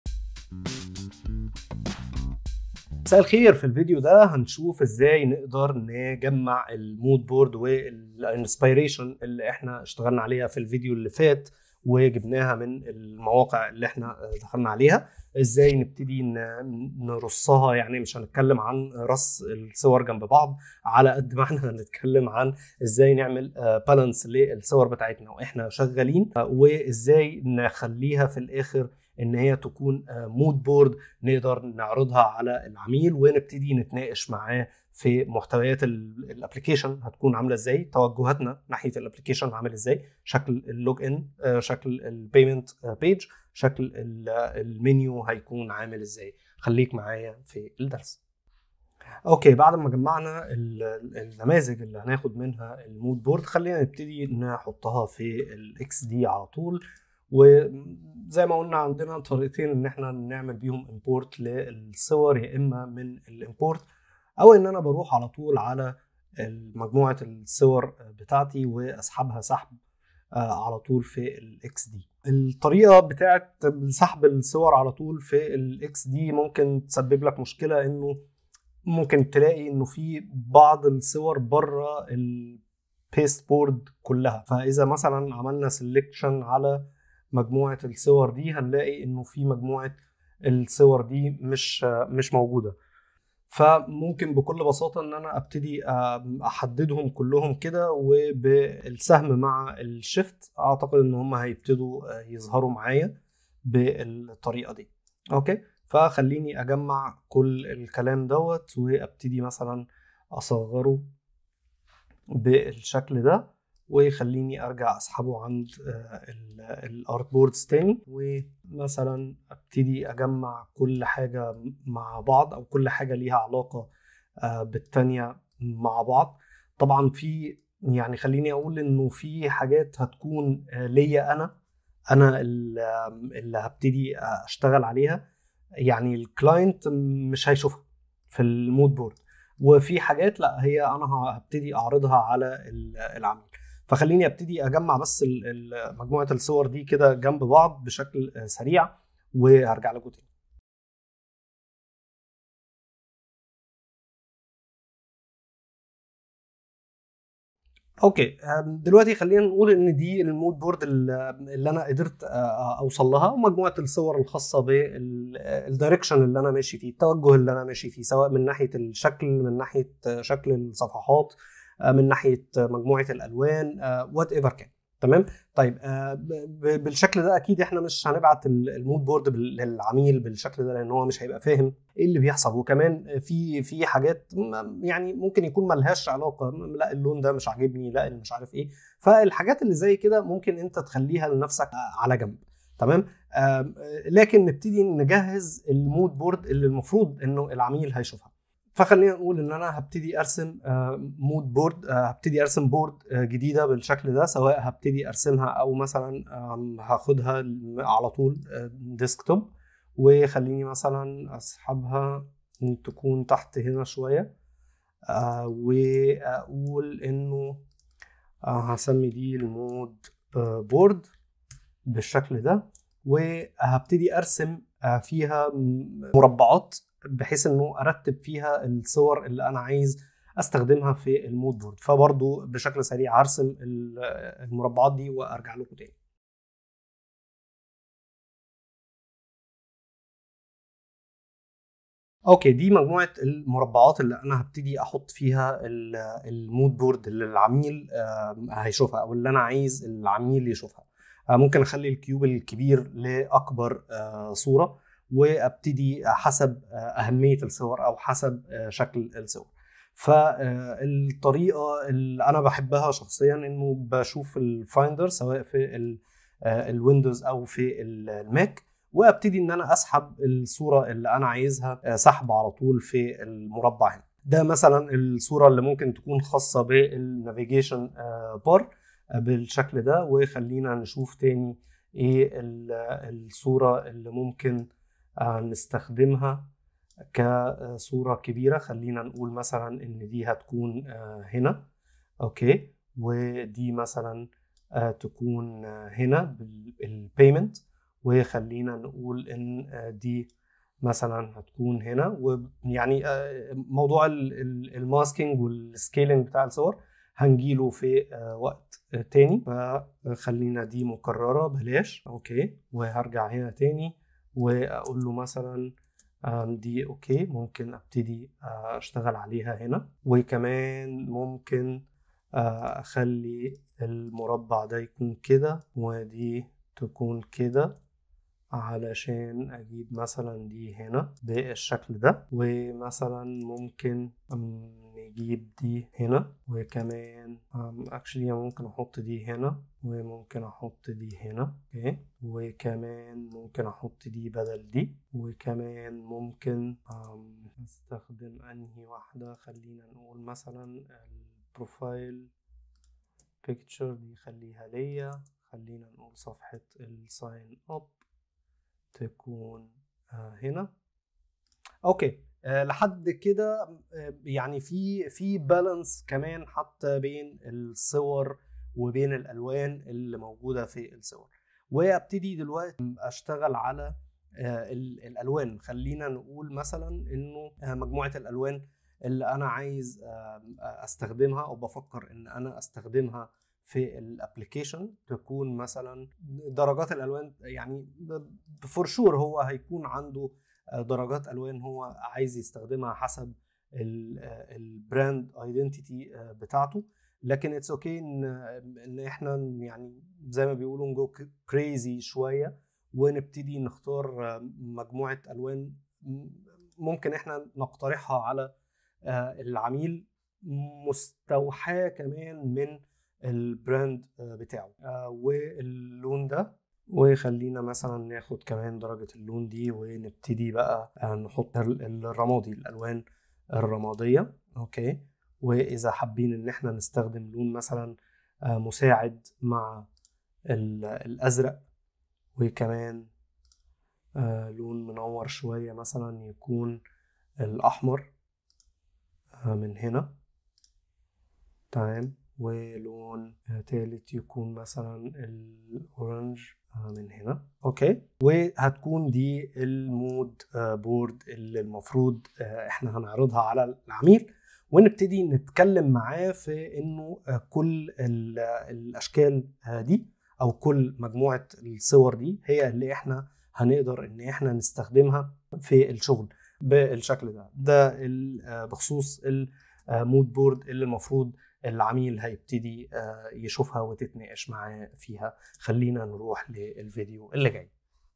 بالعربي | المودبورد الجزء الثاني XD adobexd | الدرس الخامس عشر
اوعى تخلي العميل يشوف الخطه بتاعتك وهي ملعبكة خليك محترف وقدم شغلك بطريقه توضح فيها فكرتك وخط سير الشغل